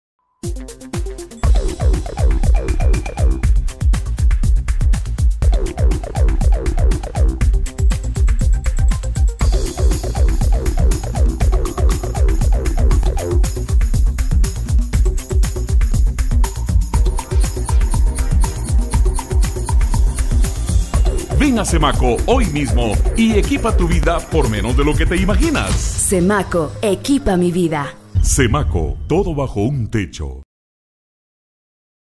CEMACO FERRETERIA.mov
Comercial realizado para la agencia Impact ( Alvaro Gamez) dirigido por Edgar Ramirez ( Guatemala) y Fotografiado por Roberto Gaitan,( Guatemala) terminamos este trabajo como a las dos de la manana, buen modelo y buen cliente. Edicion Edgar Ramirez.